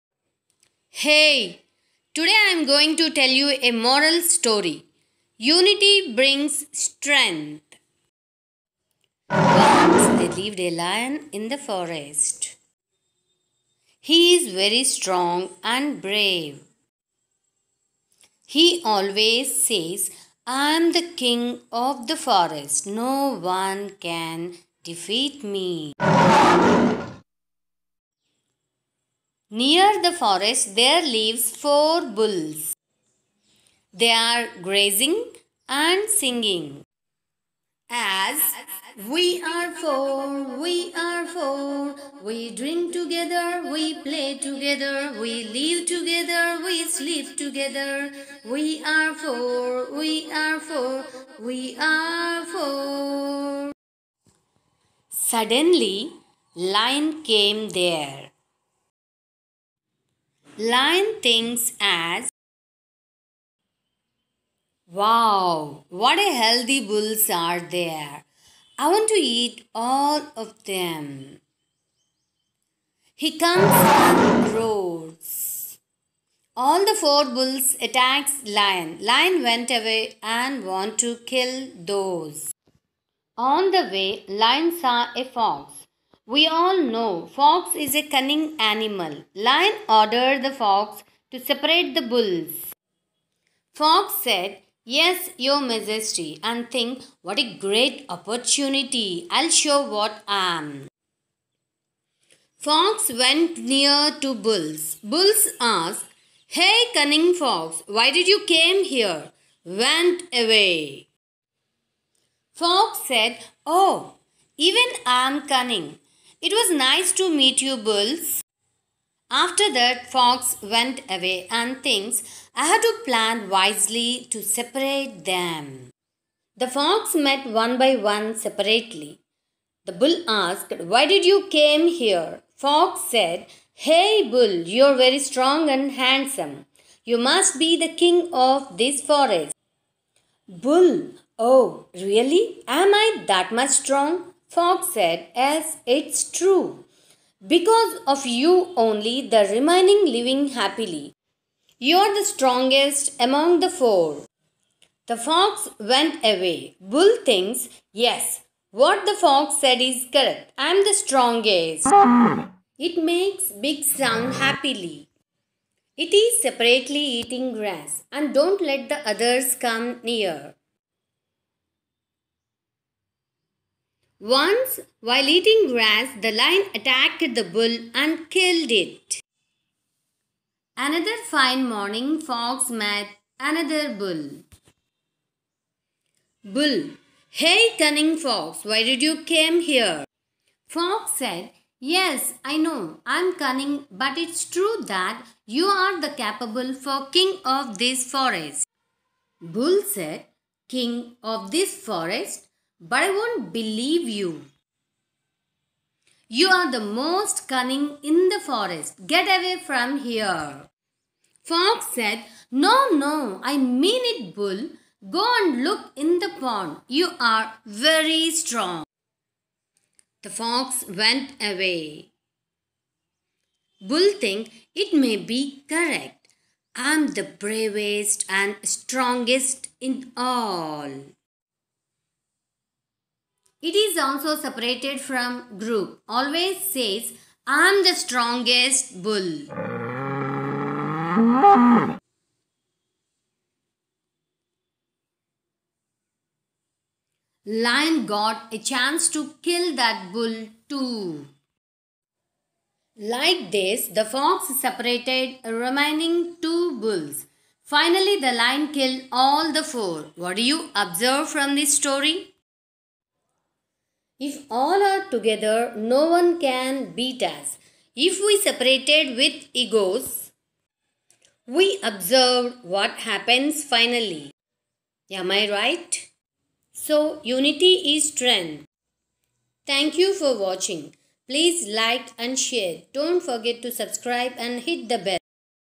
🌷Unity brings Strength🌷 - a moral story
Learns the importance of unity in regular life and implements in every moment.